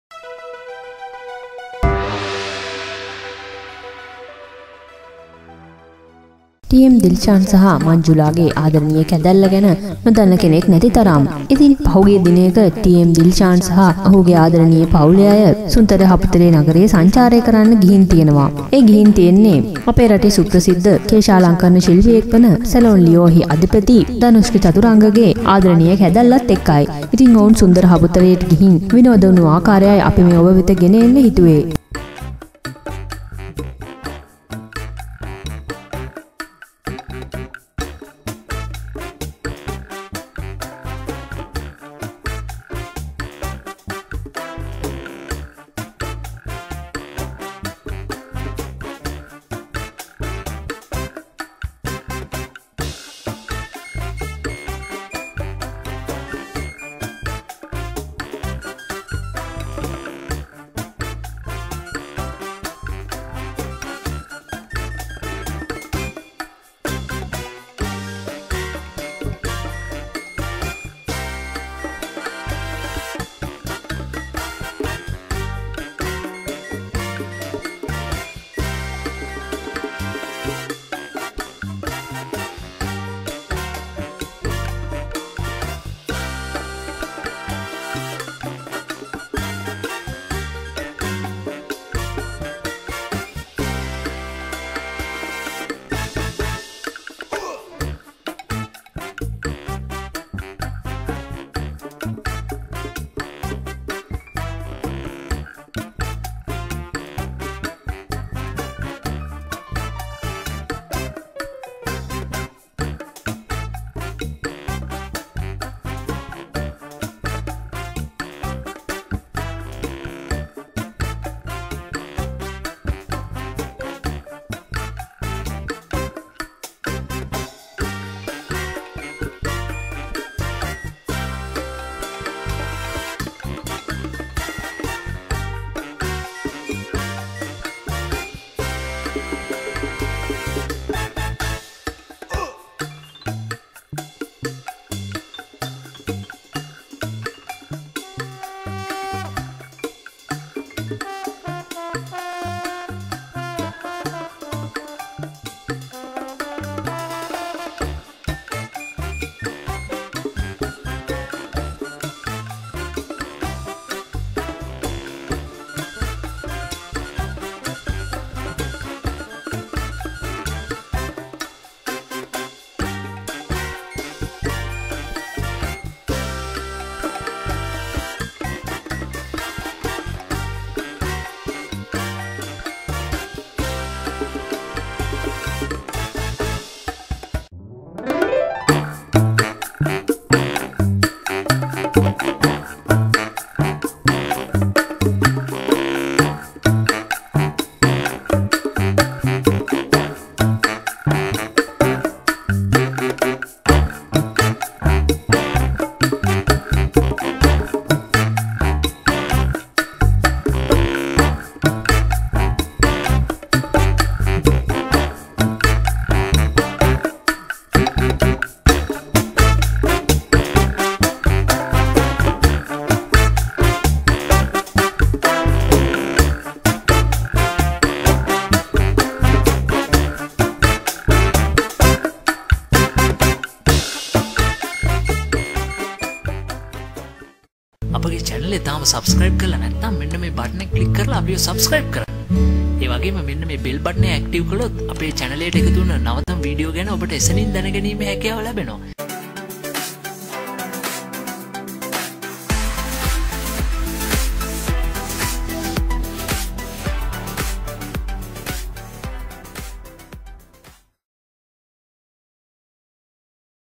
T M දිල්ශාන් ඔහුගේ ආදරණීය පවුලත් එක්ක සුන්දර හපුතලේ ගිහින් විනෝද වෙලා තිබුනේ මෙහෙමයි
T M දිල්ශාන් ඔහුගේ ආදරණීය පවුලත් එක්ක සුන්දර හපුතලේ ගිහින් විනෝද වෙලා තිබුනේ මෙහෙමයි T M dilshan Family trip to Haputhale